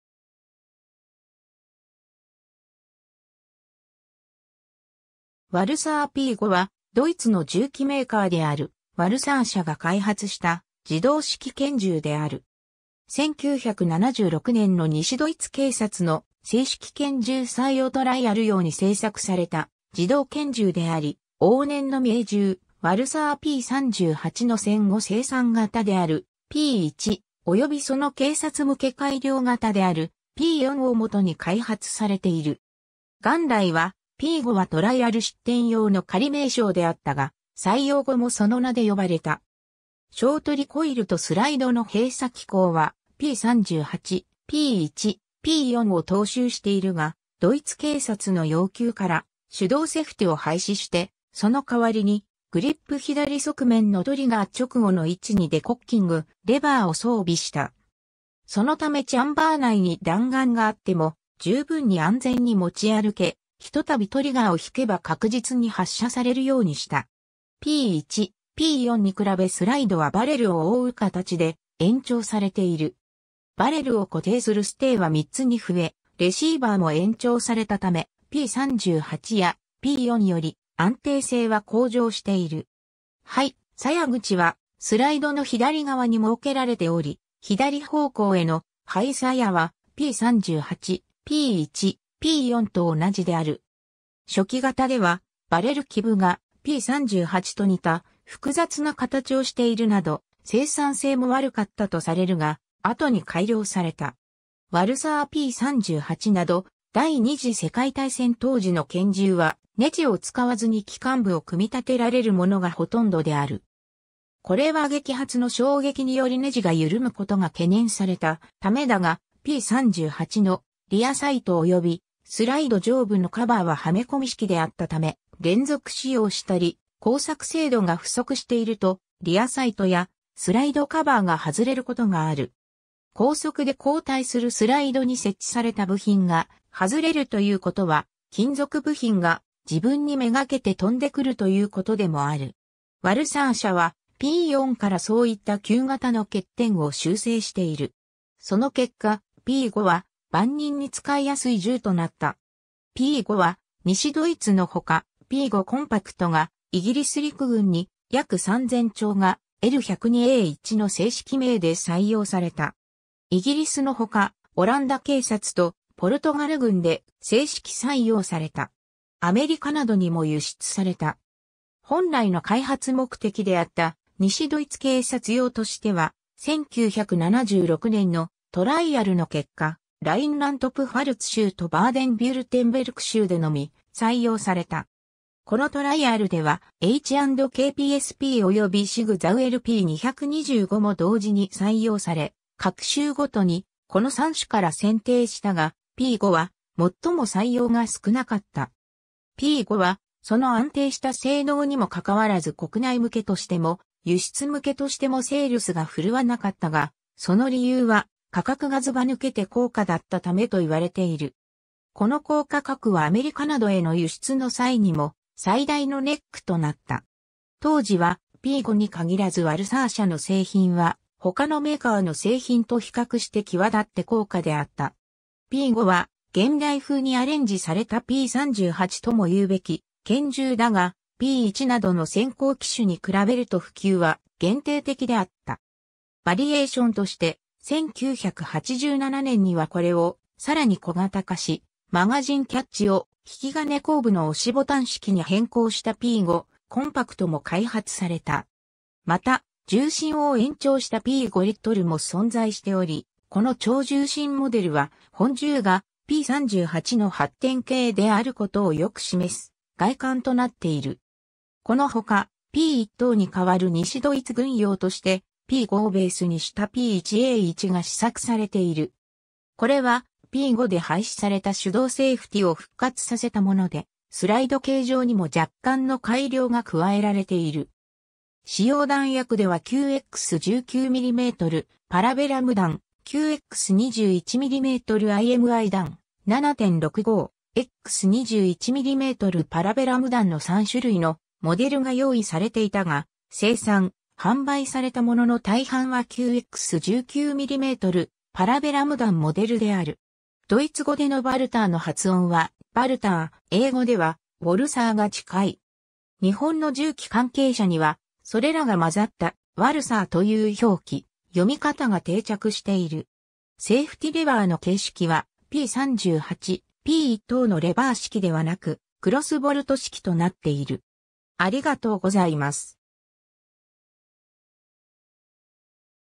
ワルサーP5
ワルサーP5 (Walther P5) はドイツの銃器メーカーであるワルサー社(ドイツ語: Walther、Carl Walther GmbH Sportwaffen)が開発した自動式拳銃である。
 1976年の西ドイツ警察の制式拳銃採用トライアル用に製作された自動拳銃であり、往年の名銃ワルサーP38の戦後生産型であるP1、およびその警察向け改良型であるP4を元に開発されている。
元来は"P5"はトライアル出典用の仮名称であったが、採用後もその名で呼ばれた。
 ショートリコイルとスライドの閉鎖機構はP38/P1/P4を踏襲しているが、ドイツ警察の要求から手動セフティを廃止して、その代わりにグリップ左側面のトリガー直後の位置にデコッキング・レバーを装備した。
そのためチャンバー内に弾丸があっても十分に安全に持ち歩け、ひとたびトリガーを引けば確実に発射されるようにした。
P1/P4に比べスライドはバレルを覆う形で延長されている。
バレルを固定するステーは3つに増え、レシーバーも延長されたためP38やP4より安定性は向上している。
排莢口はスライドの左側に設けられており、左方向への排莢はP38/P1/P4と同じである。
初期型ではバレル基部がP38と似た複雑な形をしているなど、生産性も悪かったとされるが、後に改良された。
 ワルサーP38など第二次世界大戦当時の拳銃は、ネジを使わずに機関部を組み立てられるものがほとんどである。
これは撃発の衝撃によりネジがゆるむことが懸念されたためだが、P38のリアサイト及びスライド上部のカバーははめ込み式であったため連続使用したり、工作精度が不足しているとリアサイトやスライドカバーが外れることがある。
高速で後退するスライドに設置された部品が外れるということは金属部品が自分にめがけて飛んでくるということでもある。
ワルサー社はP4からそういった旧型の欠点を修正している。
その結果P5は万人に使いやすい銃となった。
 P5は西ドイツの他、P5 コンパクトがイギリス陸軍に約3000挺が「L102A1」の制式名で採用された。
イギリスの他、オランダ警察とポルトガル軍で制式採用された。
アメリカなどにも輸出された。
 本来の開発目的であった西ドイツ警察用としては、1976年のトライアルの結果、ラインラント＝プファルツ州 とバーデン＝ヴュルテンベルク州 でのみ採用された。
このトライアルではH&K PSP(P7)及びSIG SAUER P225(P6)も同時に採用され、各州ごとにこの3種から選定したが、P5は最も採用が少なかった。
 P5はその安定した性能にもかかわらず国内向けとしても輸出向けとしてもセールスが振るわなかったが、その理由は価格がずば抜けて高価だったためと言われている。
この高価格はアメリカなどへの輸出の際にも最大のネックとなった。
当時はP5に限らずワルサー社の製品は他のメーカーの製品と比較して際だって高価であった。
P5は現代風にアレンジされたP38ともいうべき拳銃だが、P1などの先行機種に比べると普及は限定的であった。
 バリエーションとして、1987年にはこれをさらに小型化し、マガジンキャッチを引き金後部の押しボタン式に変更したP5 コンパクト(P5 COMPACT)も開発された。
また、銃身を延長したP5Lも存在しており、この長銃身モデルは本銃がP38の発展形であることをよく示す外観となっている。
 この他、P1等に代わる西ドイツ軍用としてP5をベースにしたP1A1が試作されている。
これはP5で廃止された手動セイフティを復活させたもので、スライド形状にも若干の改良が加えられている。
 使用弾薬では9x19mmパラベラム弾、9x21mm_IMI弾、7.65x21mmパラベラム弾の3種類のモデルが用意されていたが、生産・販売されたものの大半は9x19mmパラベラム弾モデルである。
 ^ ドイツ語でのWaltherの発音は「ヴァルター」、英語では「ウォルサー」が近い。
日本の銃器関係者には、それらが混ざった「ワルサー」という表記・読み方が定着している。
 ^ セイフティレバーの形式はP38/P1等のレバー式ではなく、クロスボルト式（ショットガン等で用いられている、円形のボタンを押すことでセフティのON/OFFを行う方式）となっている。